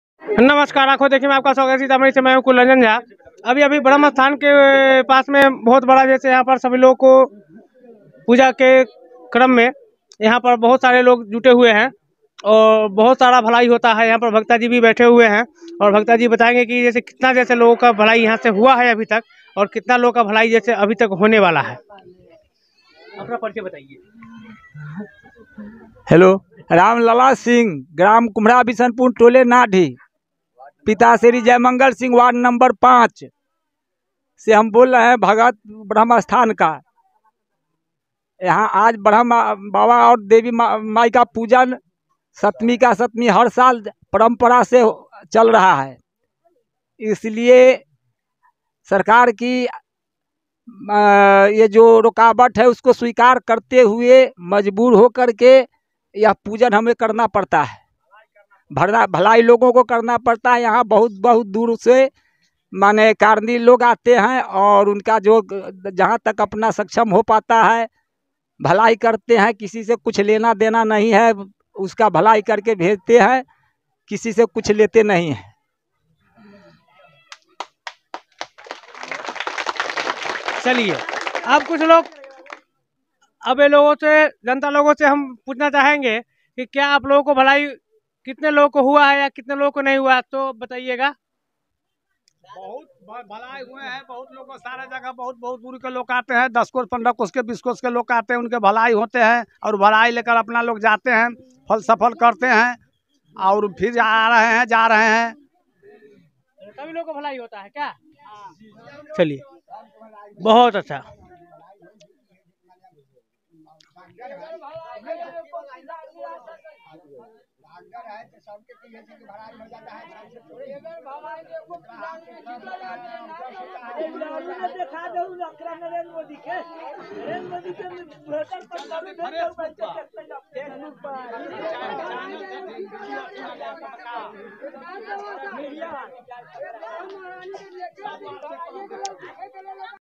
बरहमसथान कुमहार बिशनपुर से हर साल हजारों हजार के यहाँ से लोगों की भलाई होती है डुमरा सीतामढ़ी से
ankhondekhiबरहमसथान कुमहार बिशनपुर से हर साल हजारों हजार के यहाँ से लोगों की भलाई होती है डुमरा सीतामढ़ी से